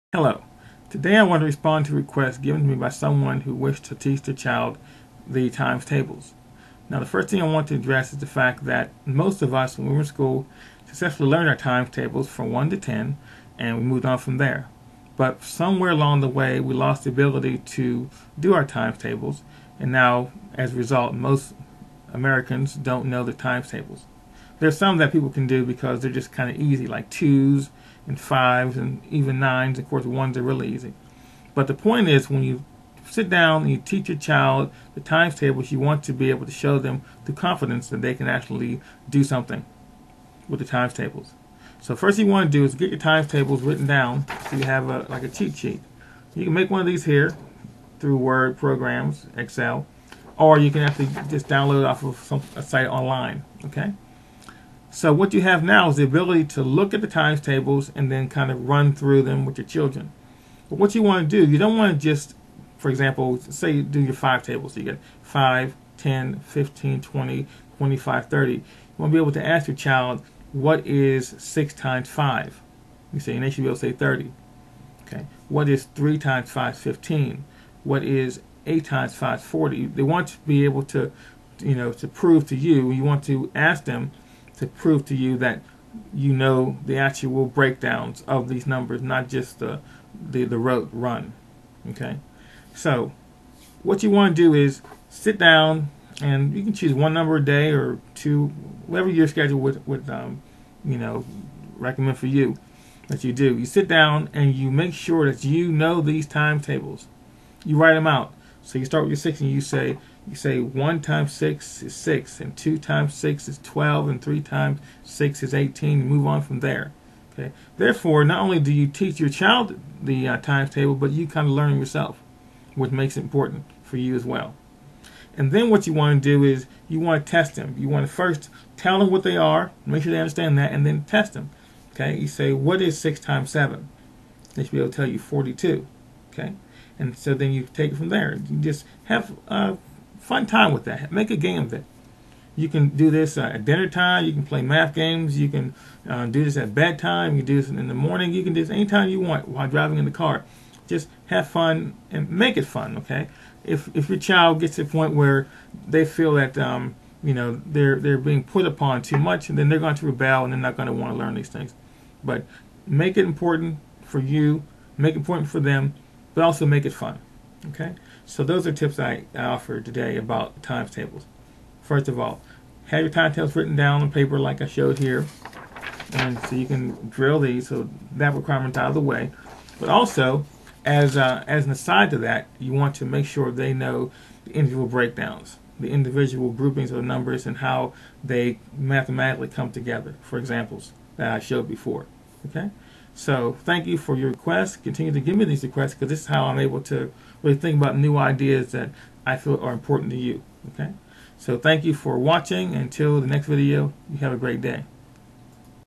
Times Tables made easy
In this video John Morrison shows you how to teach your children the times tables.